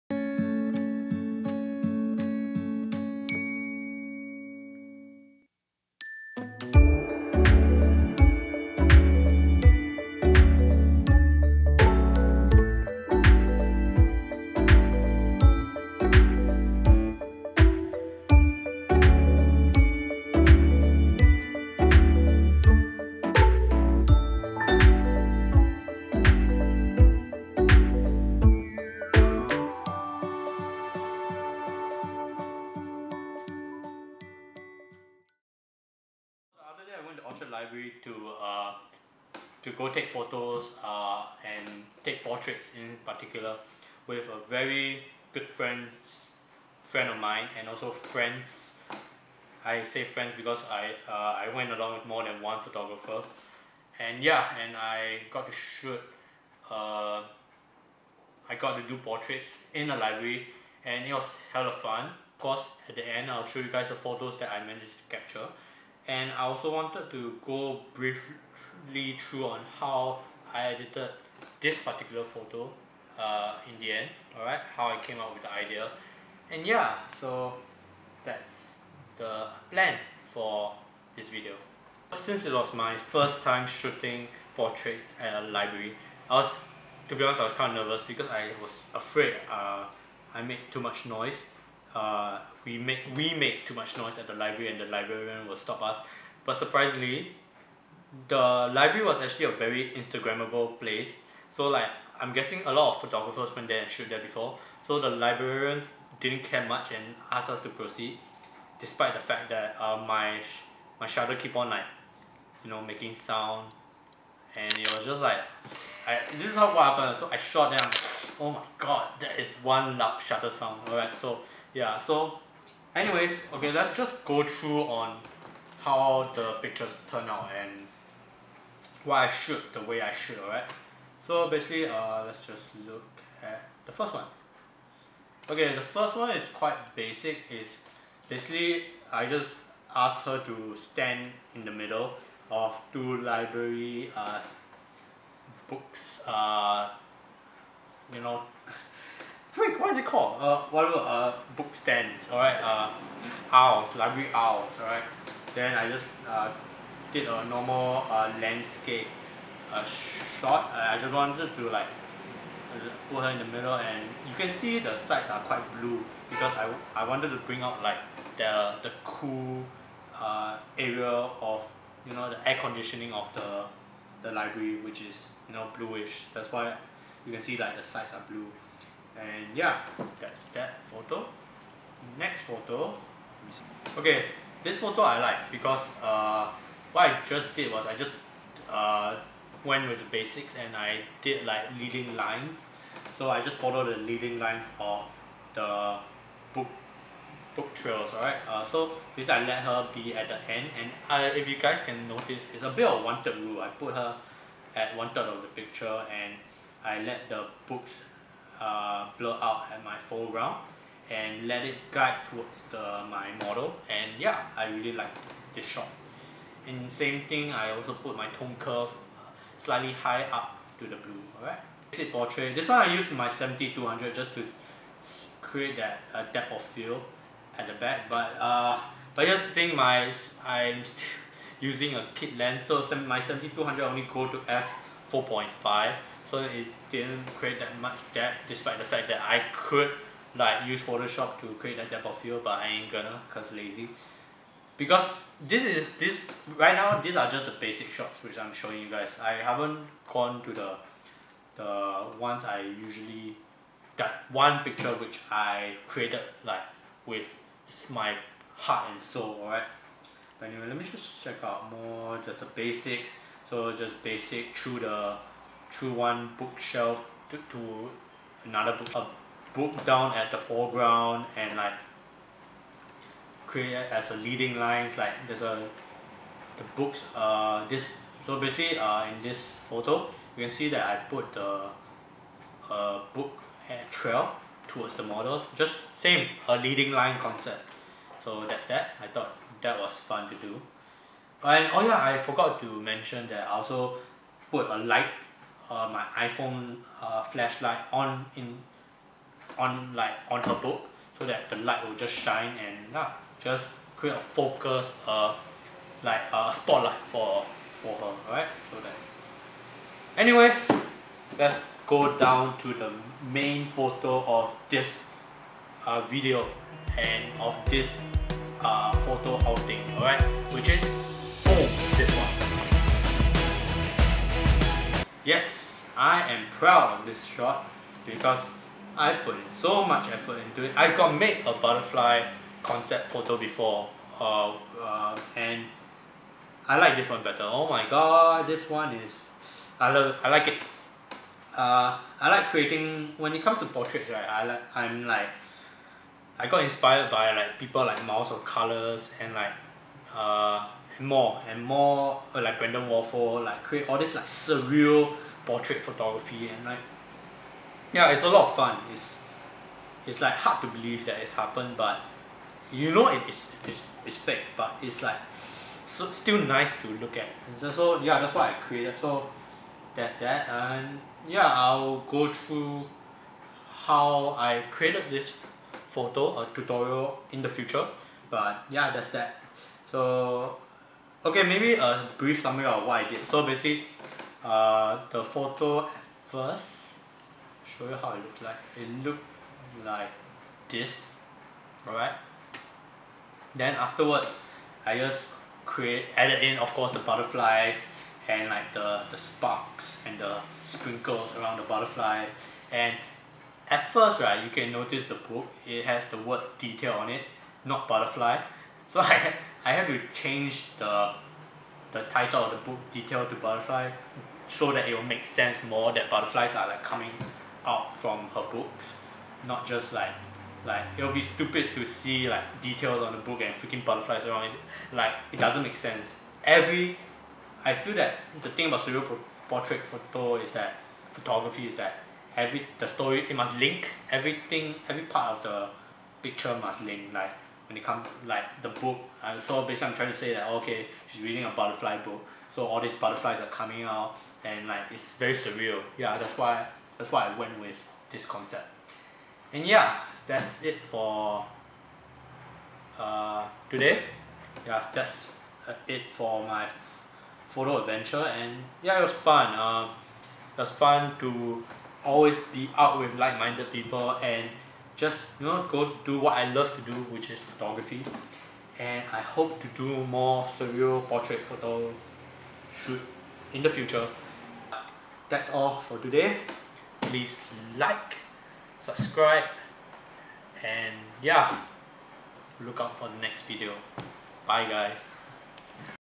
Portrait Photography in a Library!
Hi guys! Tun here with another video, this time I'll be sharing my adventure and photos which I took at the Orchard Library in Singapore. The place was such a photography area that the librarian did not even care at all ahaha. Anyways enjoy the video